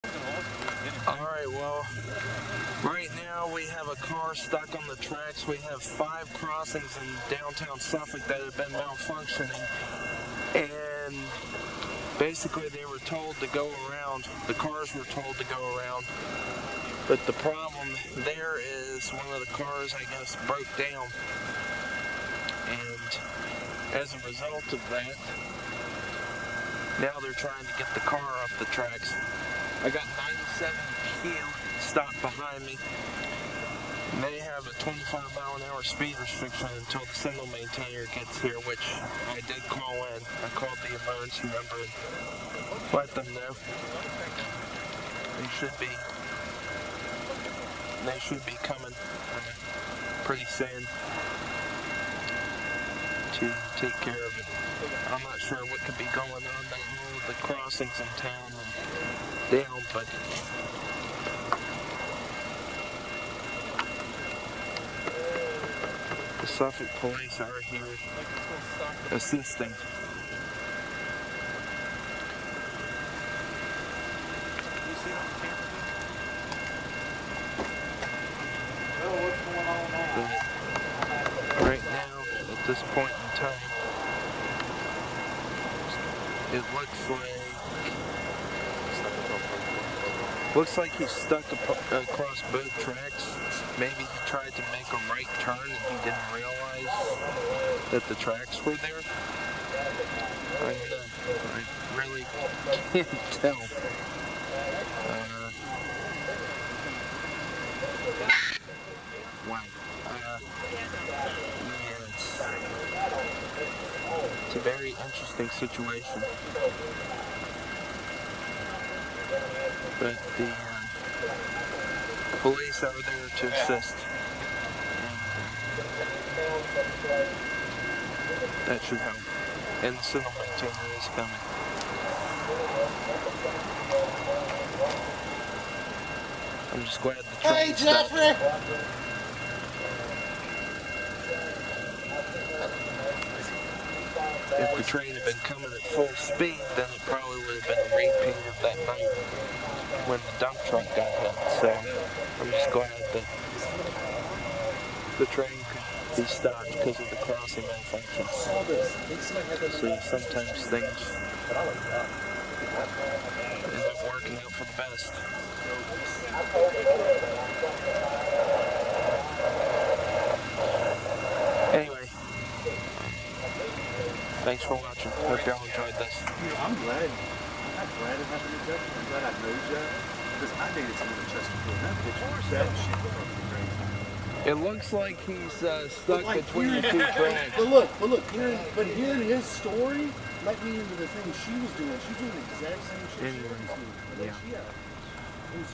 Car stuck on tracks
The gates were stuck down in downtown, all of them. I called the NS emergency number and they had the hi rail sent to come fix it. But not before not one, but TWO cars got stuck on the tracks at two different crossings since everyone was going around the lowered gates, one at the 22.7 and one at the 22.9 milepost. Shot in 4K with a Sony Handycam FDR-AX53.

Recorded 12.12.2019